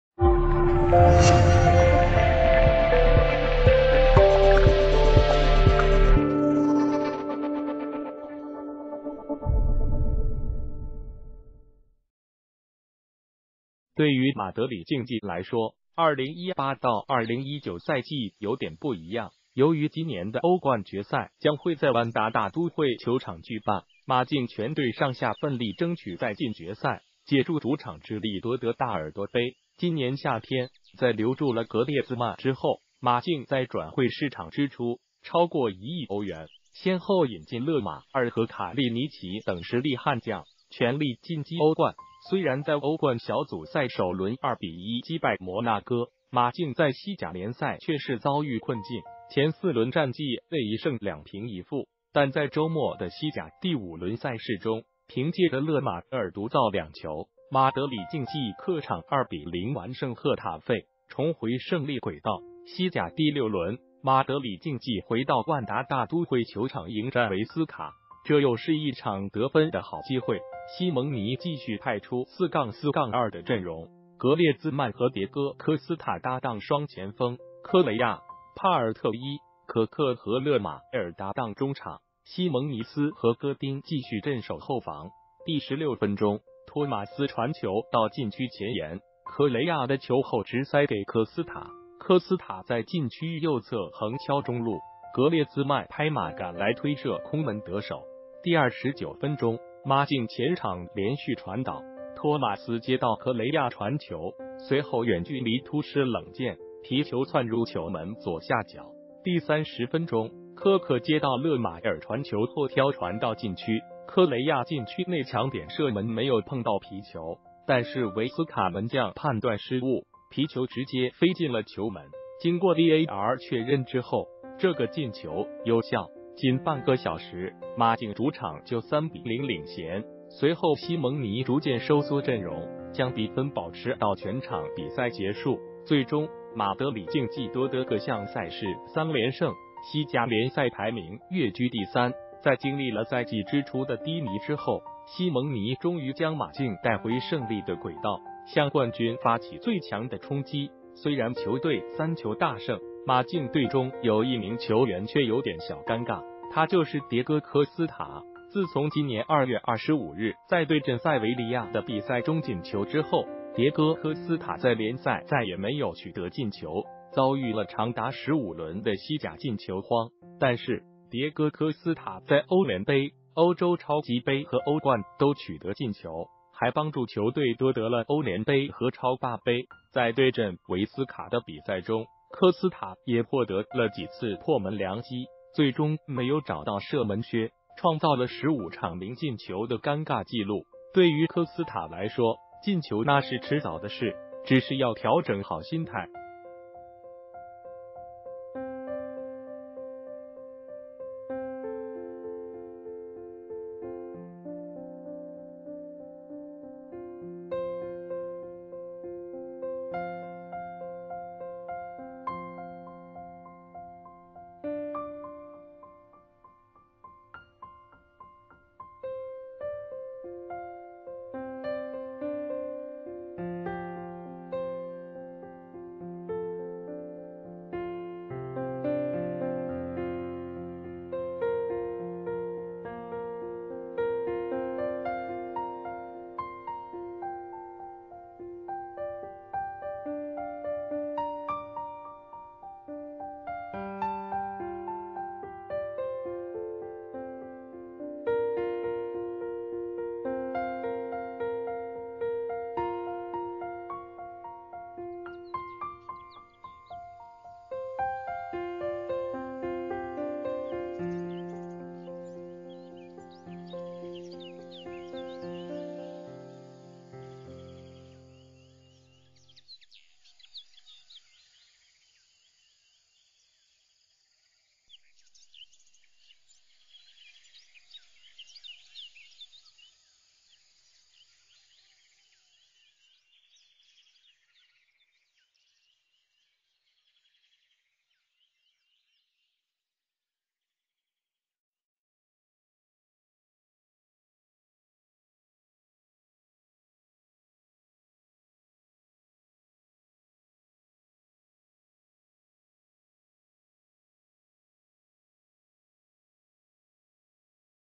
马竞3球大胜，一人却有点小尴尬，他在西甲15轮0进球
谢谢！对于马德里竞技来说，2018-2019赛季有点不一样；由于今年的欧冠决赛将会在万达大都会球场举办，马竞全队上下奋力争取再进决赛，借助主场之利夺得大耳朵杯。今年夏天，在留住了格列兹曼之后，马竞在转会市场支出超过1亿欧元，先后引进勒马尔和卡利尼奇等实力悍将，全力进击欧冠。虽然在欧冠小组赛首轮2-1击败摩纳哥，马竞在西甲联赛却是遭遇困境，前4轮战绩为1胜2平1负。但在周末的西甲第5轮赛事中，凭借着勒马尔独造两球，马德里竞技客场2-0完胜赫塔费，重回胜利轨道。西甲第6轮，马德里竞技回到万达大都会球场迎战韦斯卡，这又是一场得分的好机会。西蒙尼继续派出4-4-2的阵容，格列兹曼和迭戈-科斯塔搭档双前锋，科雷亚、帕尔特伊、科克和勒马尔搭档中场，西蒙尼斯和戈丁继续镇守后防。第16分钟，托马斯传球到禁区前沿，科雷亚得球后直塞给科斯塔，科斯塔在禁区右侧横敲中路，格列兹曼拍马赶来推射空门得手。第29分钟，马竞前场连续传导，托马斯接到科雷亚传球，随后远距离突施冷箭，皮球窜入球门左下角。第30分钟，科克接到勒马尔传球后挑传到禁区，科雷亚禁区内抢点射门没有碰到皮球，但是韦斯卡门将判断失误，皮球直接飞进了球门。经过VAR确认之后，这个进球有效。仅半个小时，马竞主场就3-0领衔，随后西蒙尼逐渐收缩阵容，将比分保持到全场比赛结束。最终，马德里竞技夺得各项赛事三连胜，西甲联赛排名跃居第三。在经历了赛季之初的低迷之后，西蒙尼终于将马竞带回胜利的轨道，向冠军发起最强的冲击。虽然球队3球大胜，马竞队中有一名球员却有点小尴尬，他就是迭戈-科斯塔。自从今年2月25日在对阵塞维利亚的比赛中进球之后，迭戈-科斯塔在联赛再也没有取得进球，遭遇了长达15轮的西甲进球荒。但是，迭戈-科斯塔在欧联杯、欧洲超级杯和欧冠都取得进球，还帮助球队夺得了欧联杯和超霸杯。在对阵韦斯卡的比赛中，科斯塔也获得了几次破门良机，最终没有找到射门靴，创造了15场0进球的尴尬纪录。对于科斯塔来说，进球那是迟早的事，只是要调整好心态。